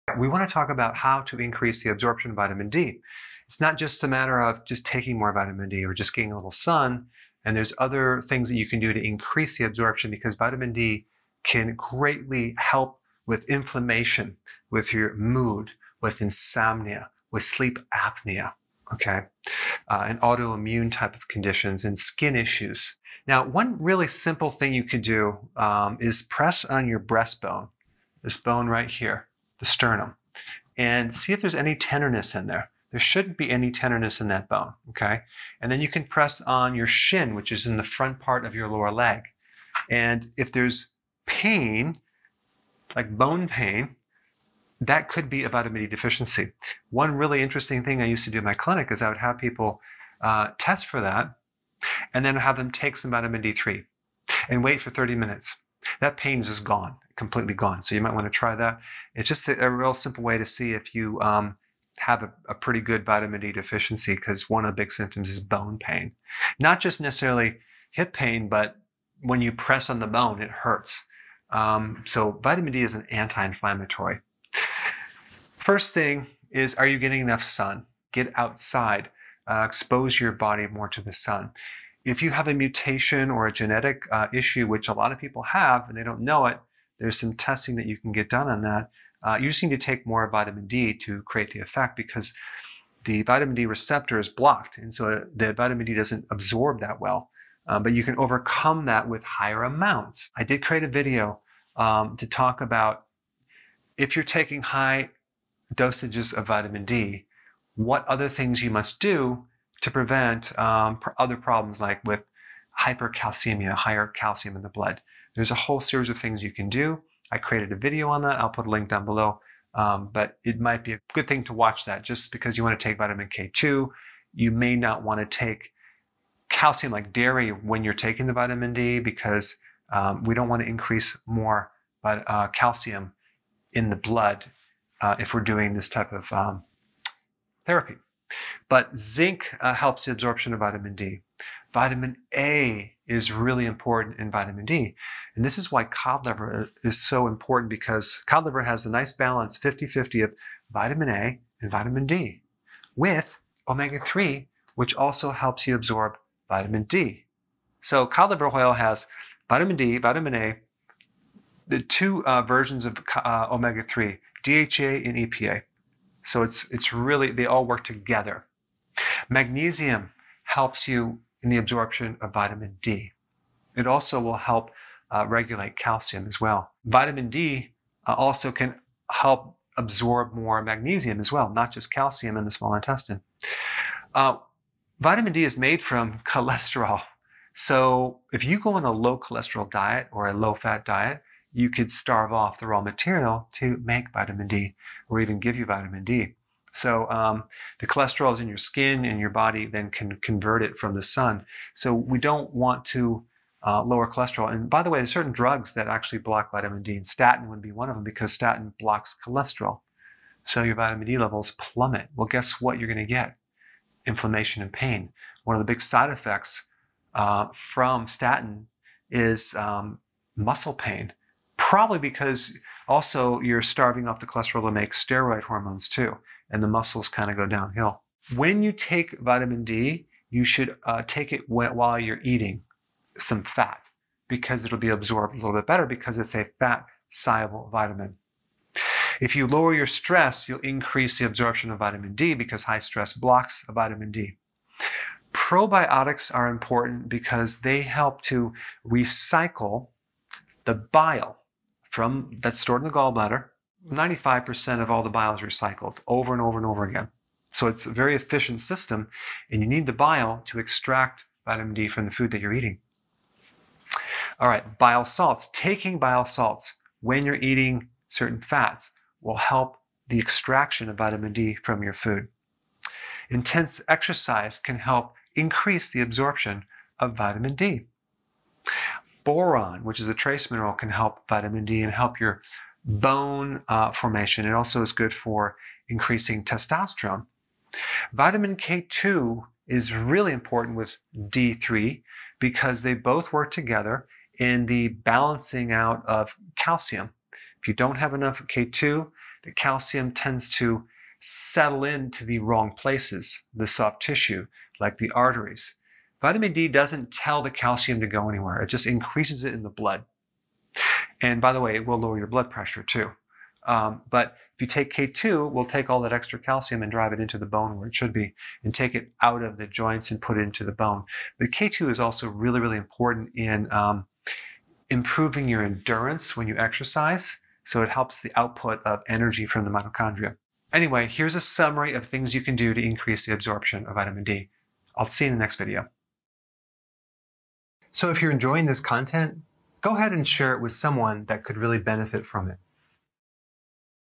How to Increase Your Vitamin D Absorption?
There are other things you can do besides getting more sun to help increase the absorption of vitamin D. Find out what they are.

Timestamps
0:00 Increase vitamin D absorption
0:10 Benefits of vitamin D
0:22 How to tell if you have a vitamin D deficiency
1:15 How to increase vitamin D absorption

In this video, we’re going to talk about how to increase the absorption of vitamin D from the sun. One simple way to test if you may have a vitamin D deficiency is to press on your sternum or shin, and if there is bone pain, you may be deficient.

Vitamin D benefits:
1. It can help decrease inflammation
2. It can help improve your mood
3. It can help increase sleep quality
4. It can help with autoimmune conditions
5. It can help with skin issues

How to increase vitamin D absorption:
• Get outside and get more sun
• Take more vitamin D
• Take zinc
• Take vitamin A
• Take magnesium
• Avoid low-fat diets or low-cholesterol diets
• Lower your stress
• Take probiotics
• Take bile salts
• Do intense exercise
• Take boron
• Take vitamin K2
• Get plenty of omega-3 fatty acids

I hope this helps you better understand how to increase your absorption of vitamin D.